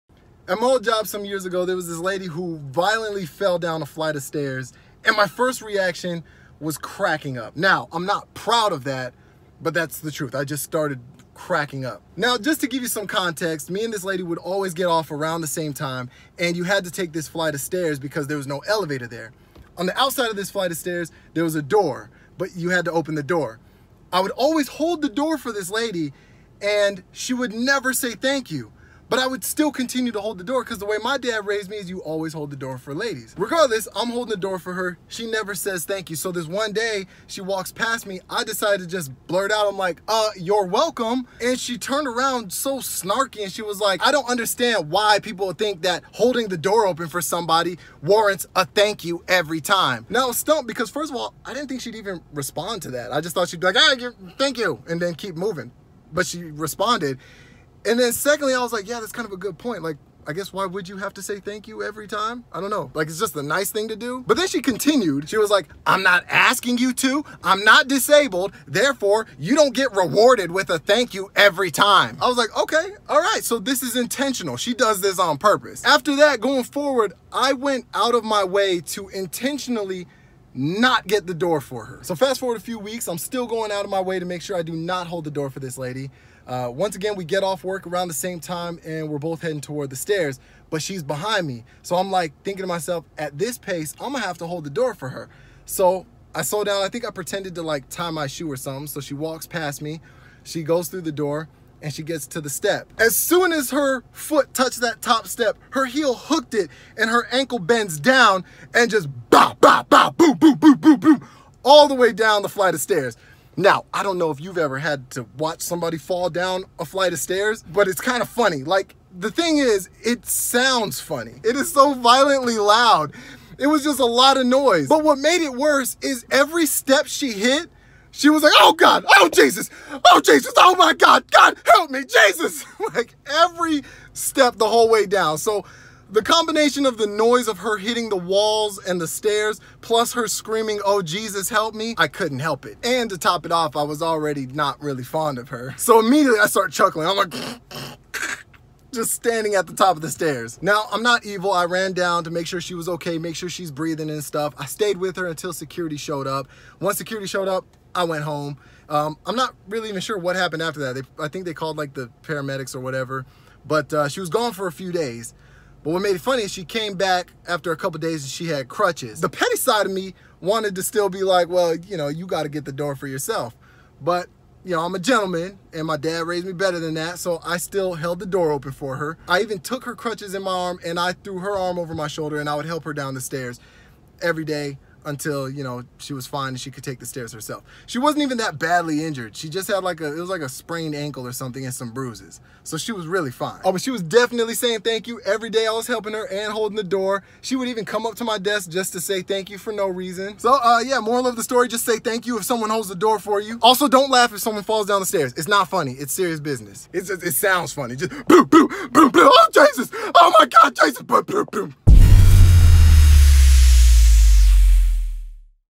She Fell Down The Stairs!
True story of the time one of my co-workers fell and kissed every stair step on the way down a staircase face first!